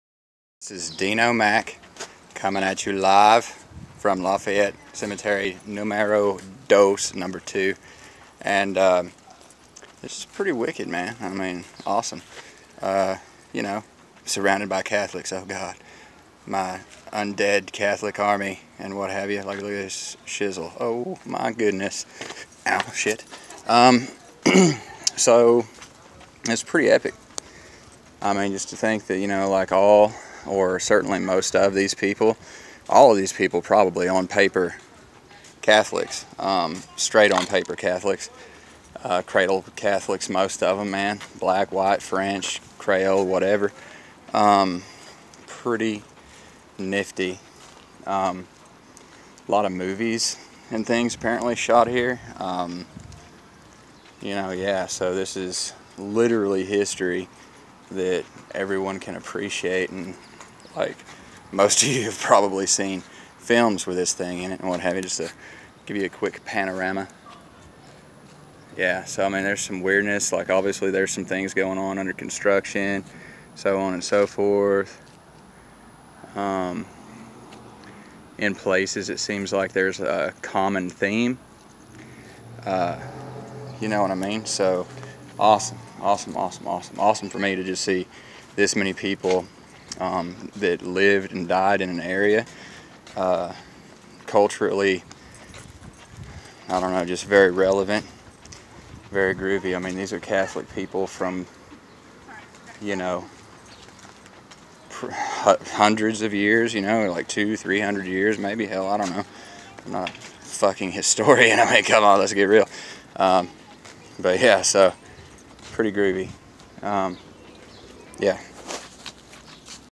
Lafayette cemetery vlog
Me ranting in the Lafayette cemetery located in the lovely New Orleans.